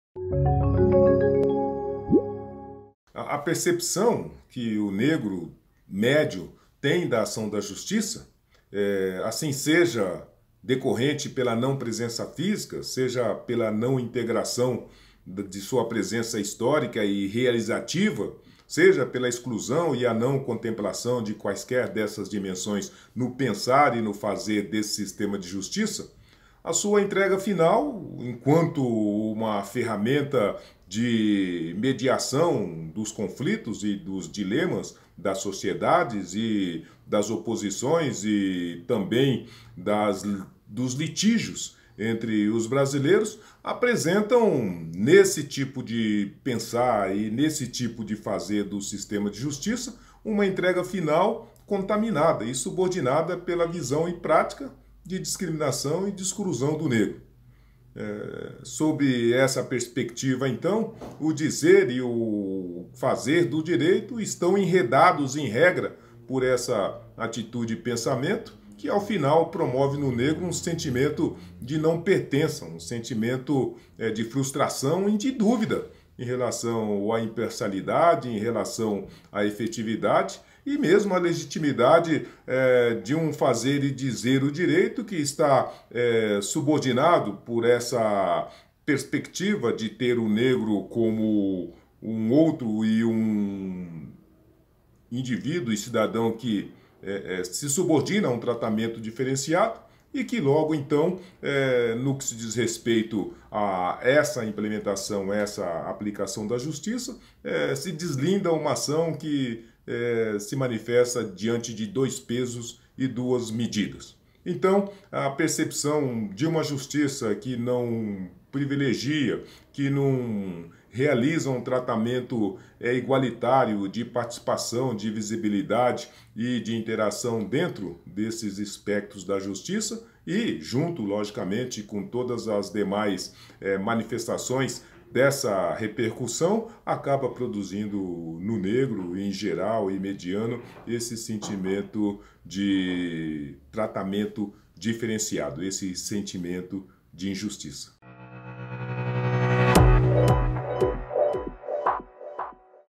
Advogado fala sobre discriminação de negros em decisões
Uma pesquisa de perfil demográfico dos magistrados brasileiros do CNJ mostrou que o número de magistrados negros e negras na Justiça é baixíssimo: na Justiça Estadual, Federal e do Trabalho, por exemplo, a quantidade de juízes negros não passa de 1% em cada ramo. Já o número de magistrados brancos, ocupa a maior faixa entre os perfis "amarelo", "pardo", "preto" e "branco".